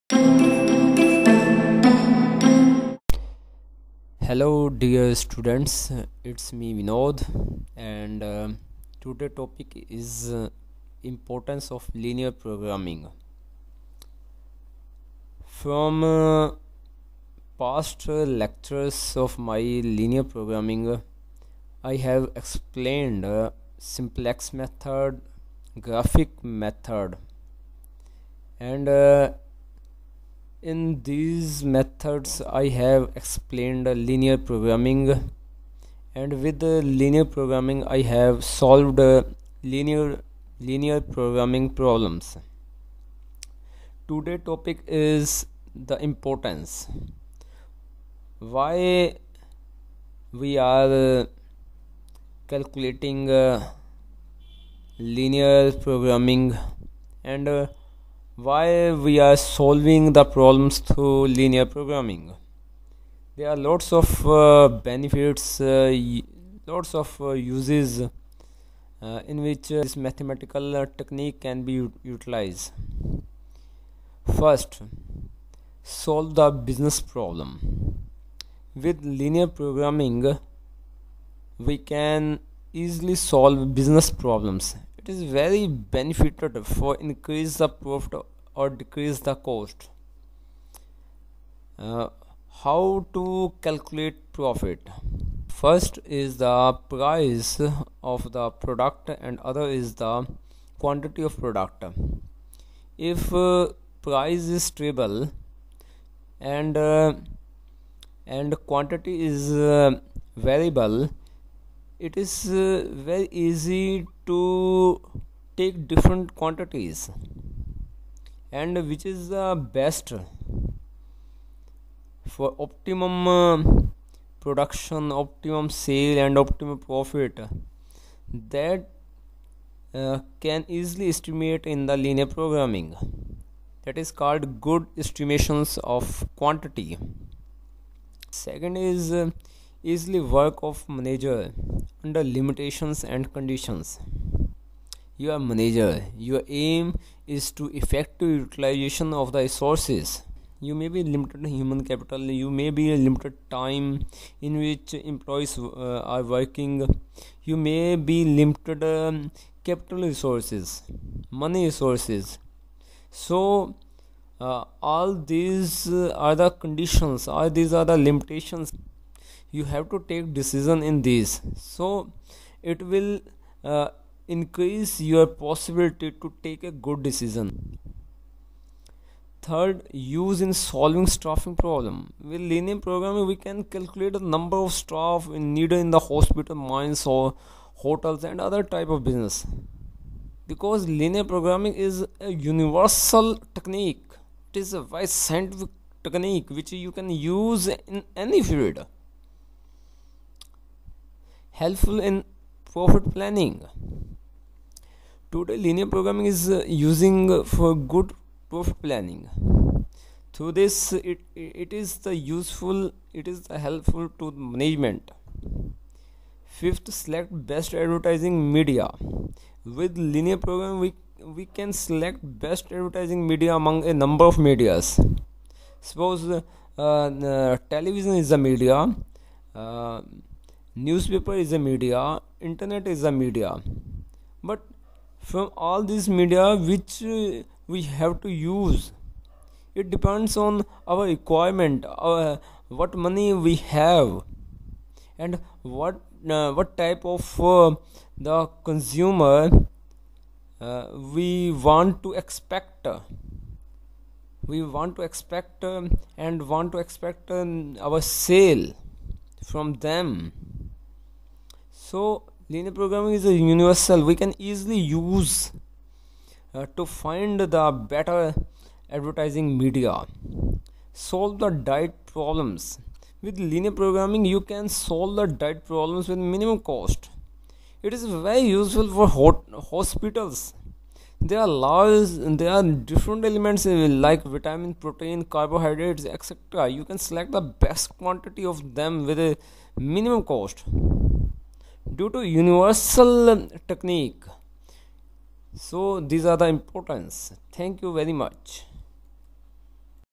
Linear Programming Importance - Part 1
In this lecture, I have include some basic points which show the importance of linear programming. In future, I will make the video to explain importance of linear programming more deeply. For watching all maths video free, please